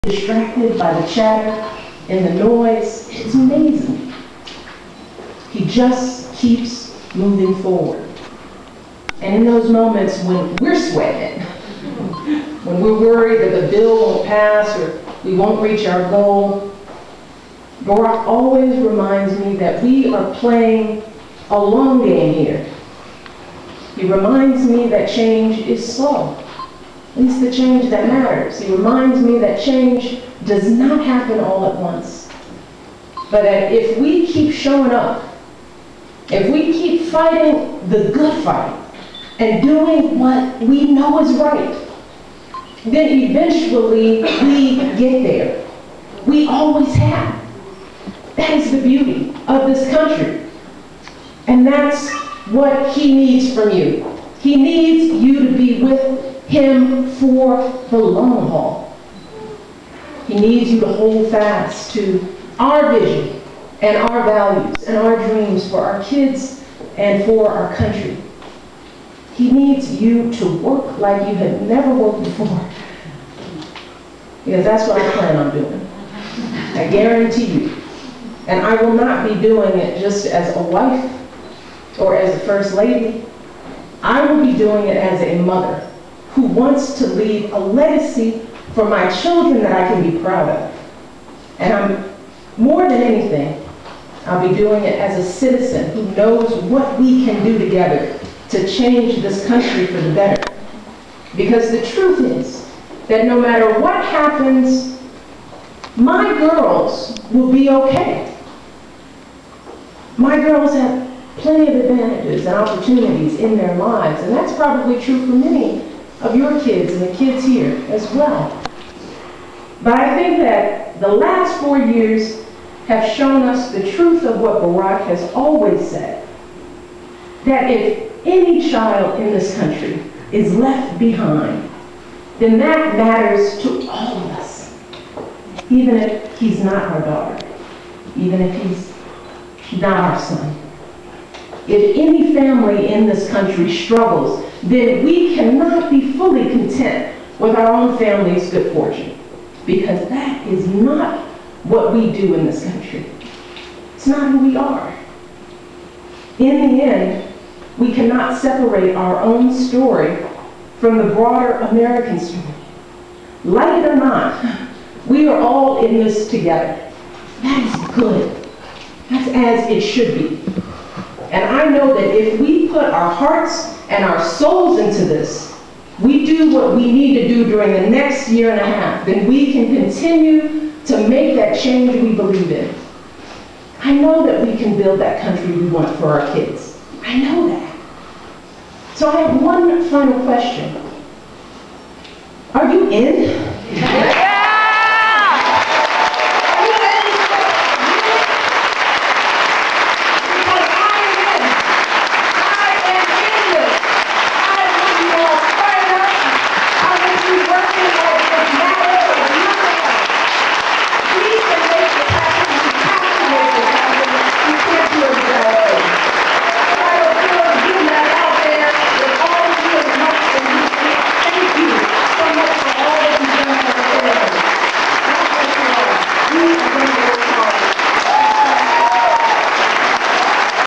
First Lady Visits --Speech
The First Lady visited Park City Utah. This is the tail end of her speech, my friend got the rest of that. When she uploads it I'll link it here.